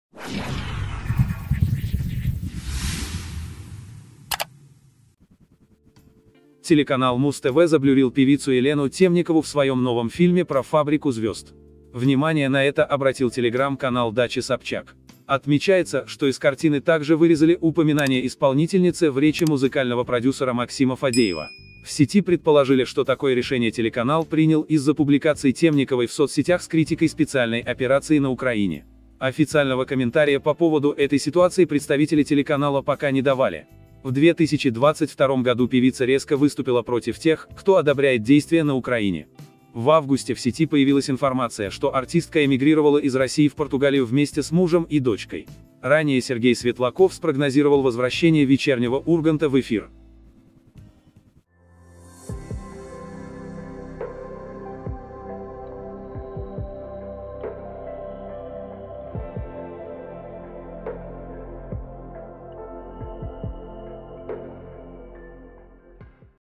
Муз-ТВ стер Елену Темникову из фильма про «Фабрику звезд»
Муз-ТВ стер осудившую СВО Елену Темникову из фильма про «Фабрику звезд».